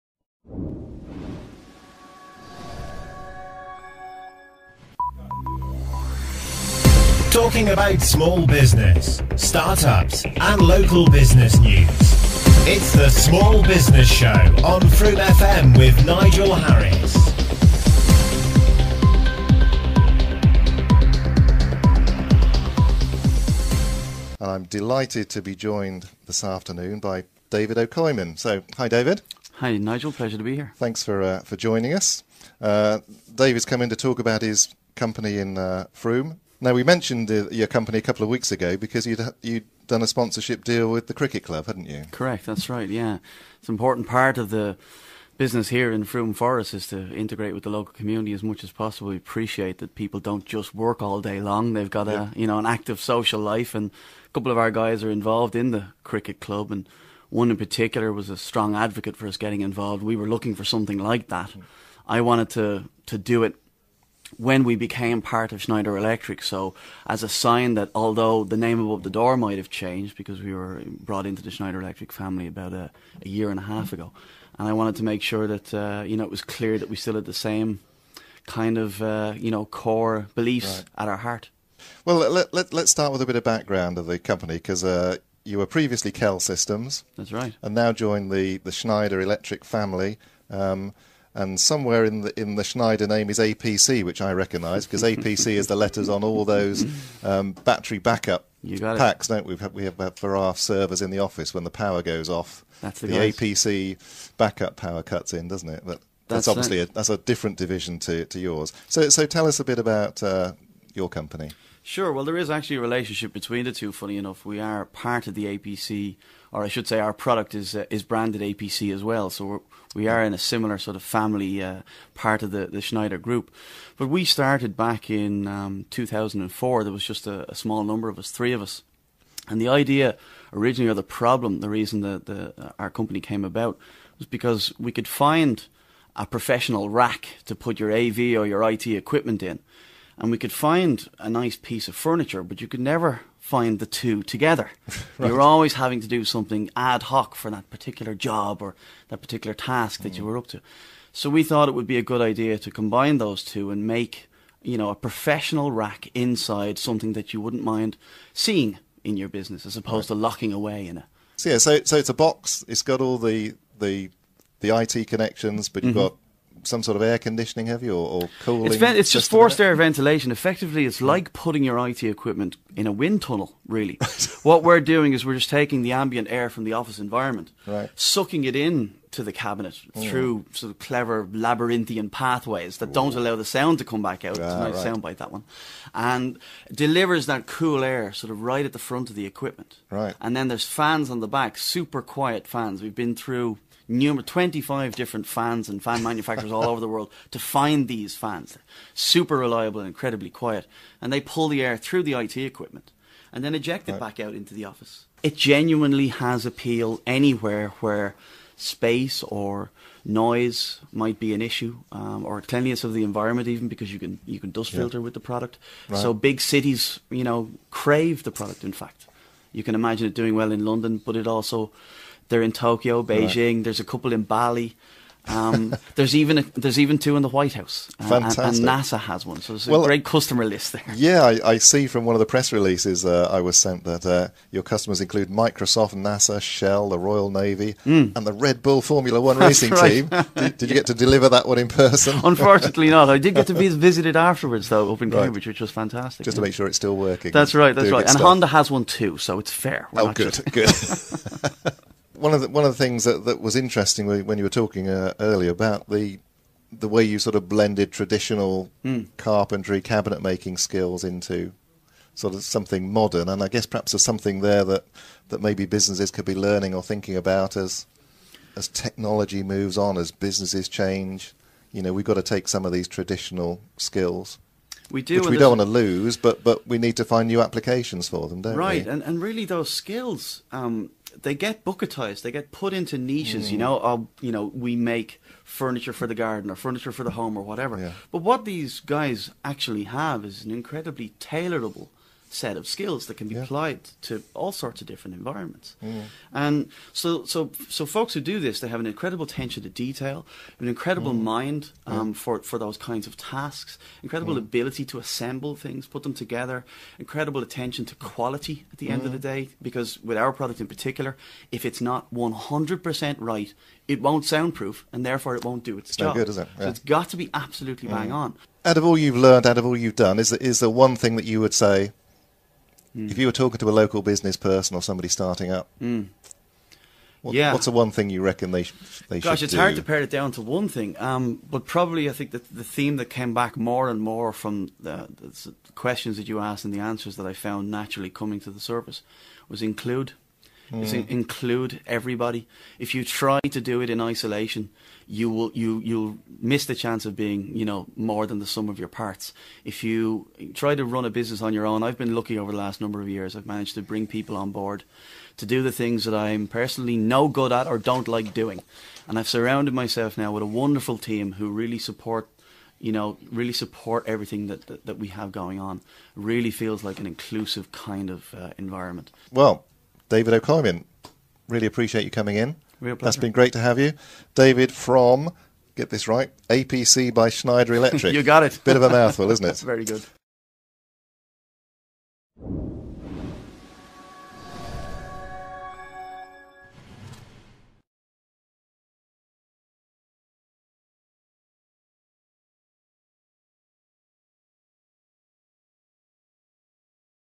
David O'Coimin on Frome FM
David O'Coimin, Solution Offer Manager for Schneider Electric and one of the founders of the NetShelter CX talks to Frome FM For more information please